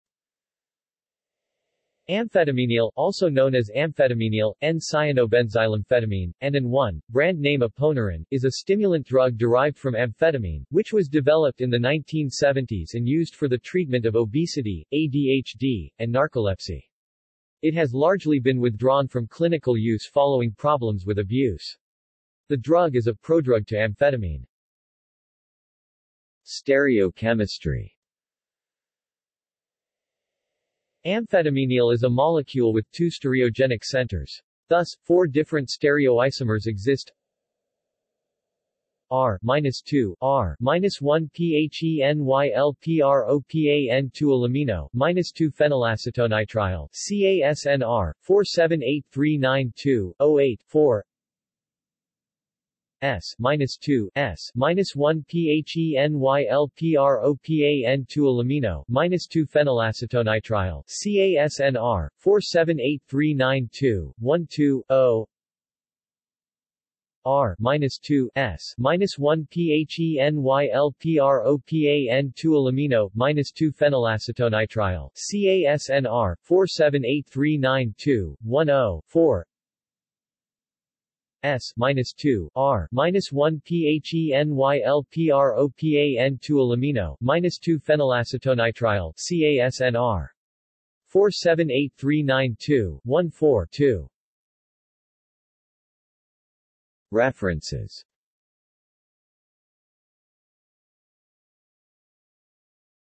Amfetaminil | Wikipedia audio article
This is an audio version of the Wikipedia Article:
Amfetaminil

SUMMARY
Amfetaminil (also known as amphetaminil, N-cyanobenzylamphetamine, and AN-1; brand name Aponeuron) is a stimulant drug derived from amphetamine, which was developed in the 1970s and used for the treatment of obesity, ADHD, and narcolepsy. It has largely been withdrawn from clinical use following problems with abuse. The drug is a prodrug to amphetamine.